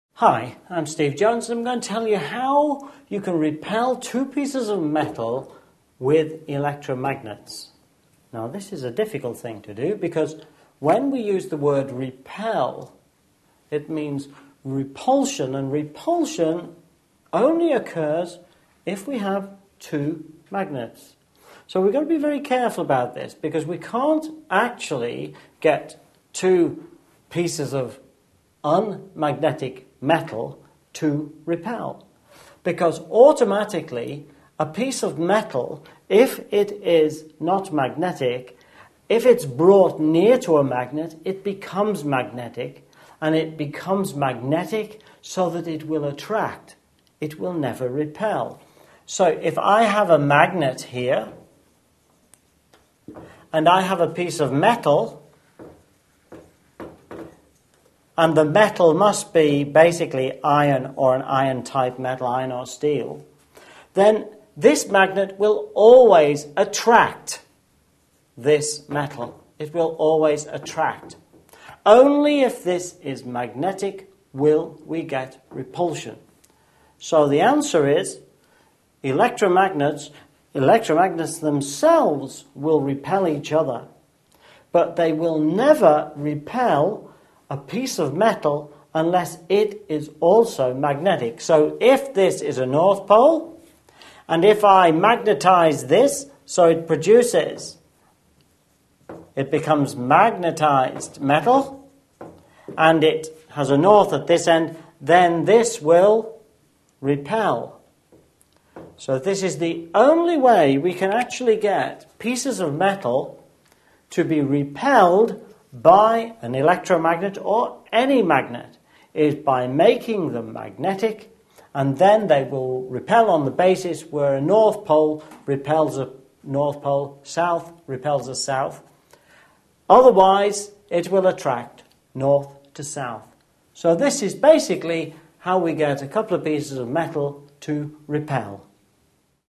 How to Repel 2 Pieces of Metal With Electromagnets
How to Repel 2 Pieces of Metal With Electromagnets. Part of the series: Physics & Electromagnetism. In order to repel two pieces of metal, two magnets are needed because electromagnets will only repel each other. Discover why electromagnets can't repel two pieces of metal with help from a science teacher in this free video on electromagnets and physics.